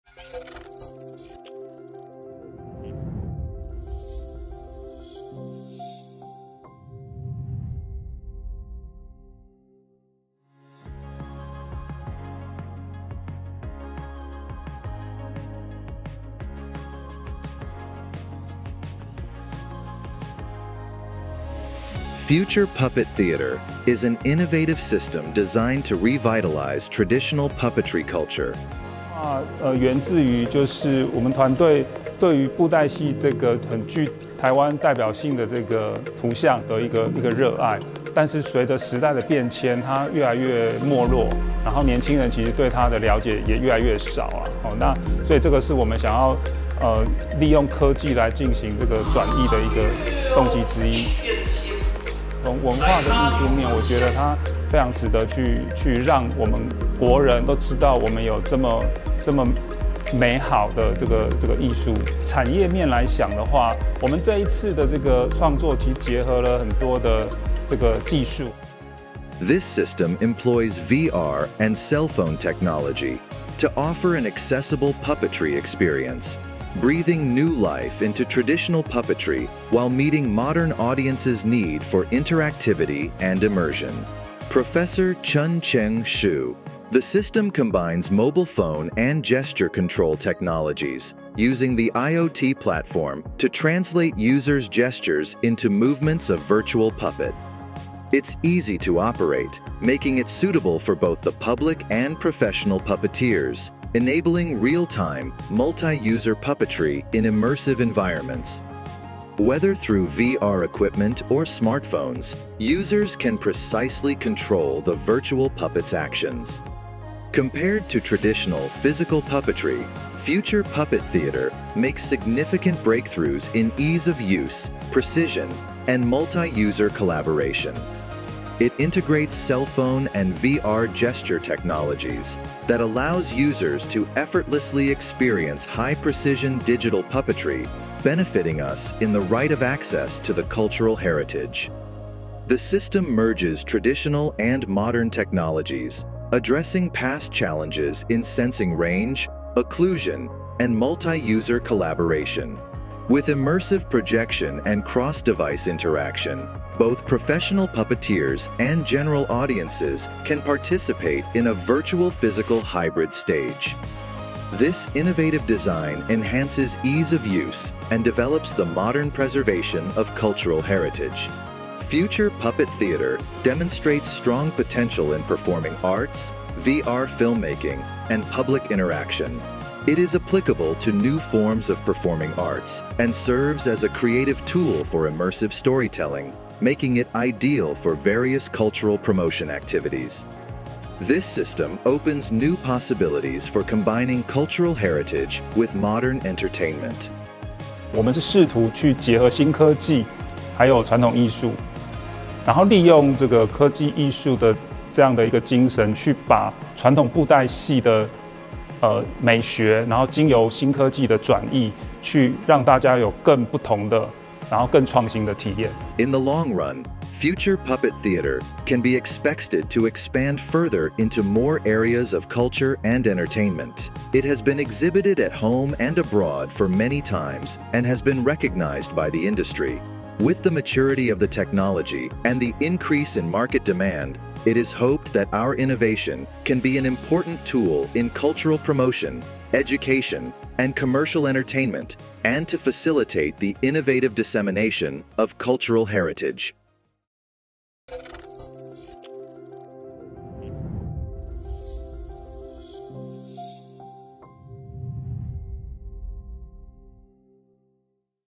2024亮點技術影片：未來偶戲劇場：基於多螢互動之沉浸式多人即時操偶系統
「Future Puppet Theater」是一款結合手機與手勢操控技術的多螢互動沉浸式即時操偶系統，通過IoT平台將數據轉化為虛擬人偶的動作。系統提供VR與手機操偶功能，實現多人共享的虛實結合劇場。透過VR手勢追蹤與手機感測器，使用者可以綁定並控制虛擬戲偶，讓大眾能透過科技輕鬆體驗操偶的樂趣。

2024台灣創新技術博覽會-未來科技館 (2024 FUTEX)
活動時間：10/17-10/19 上午9時30分至下午5時30分（最後一日(10/19)至下午4時30分）
活動地點：台北世貿一館